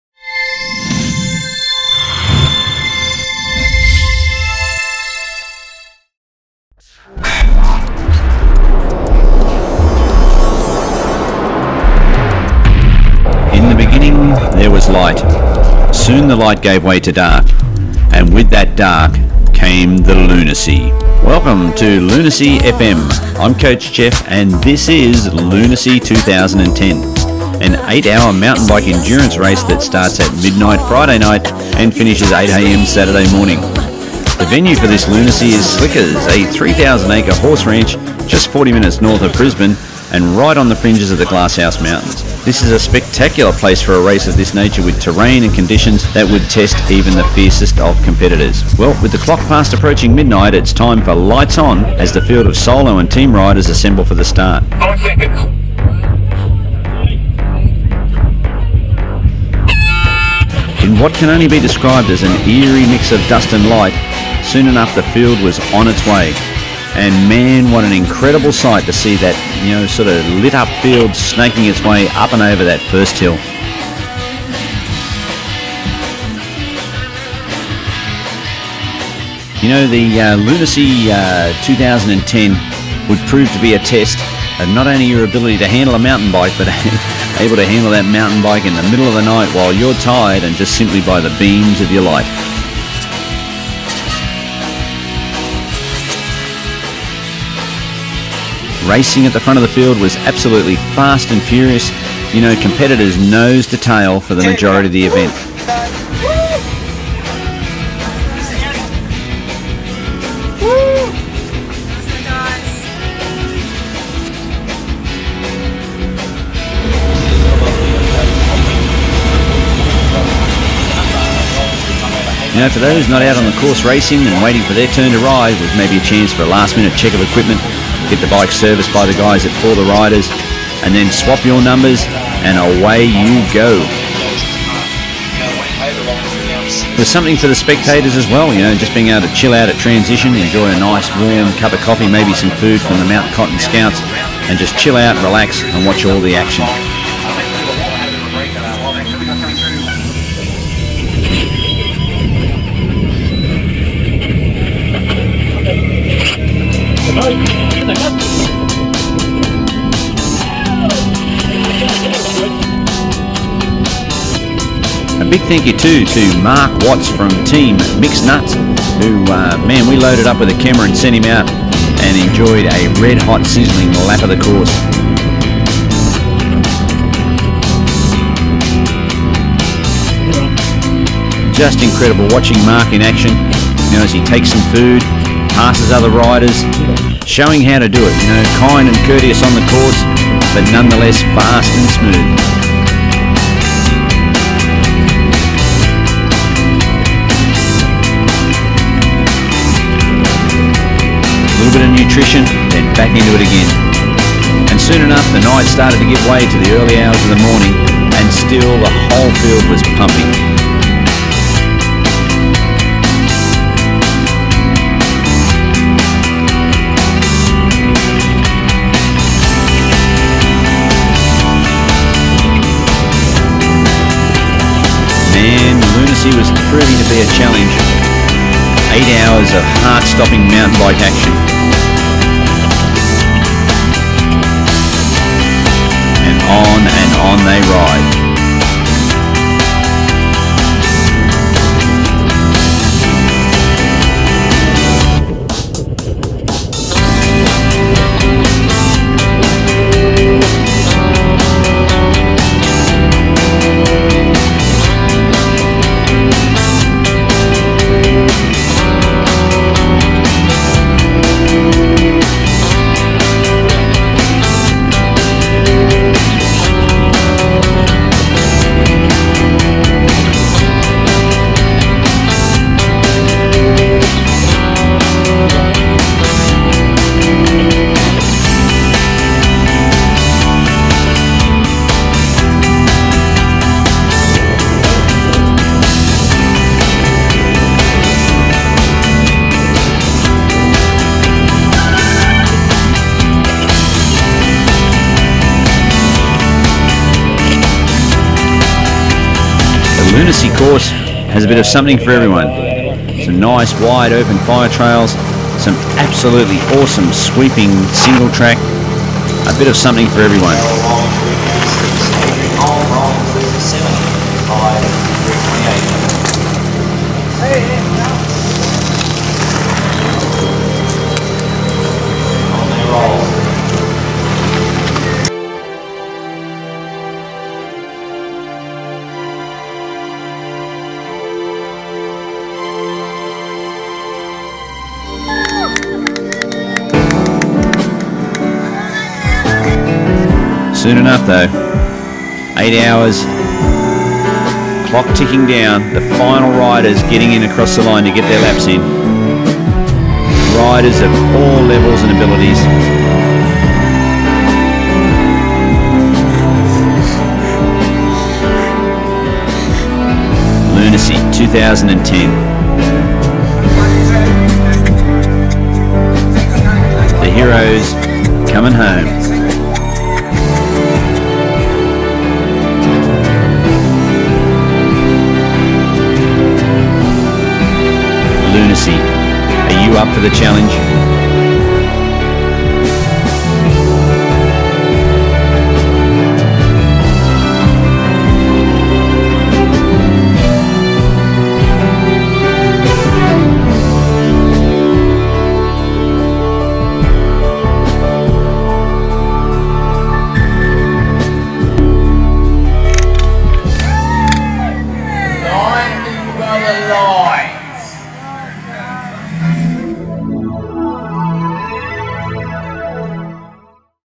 LunarC 8 Hour Mountain Bike Race - Midnight To Eight
LunarC 8hr: 15th & 16th January, 2010

You have dared to venture to the Dark Side. Before you read on... decide... are you ready for the madness of the 8hr LunarC Enduro?

LunarC is not your average mountain bike event and it's not for the faint hearted. Starting at Midnight Friday and finishing at 8am on Saturday morning LunarC turns mountain biking on it's head as competitors turn to the Dark Side for adventure with just a touch of Wickedness.

Held at Slickers, just 40 minutes north of Brisbane the LunarC 8hr throws up an all new challenge for solo riders and teams of 2 or 3. This is an awesome course for a night event so get ready for a night of madness and insanity as we worship the night in Brisbane's all new LunarC!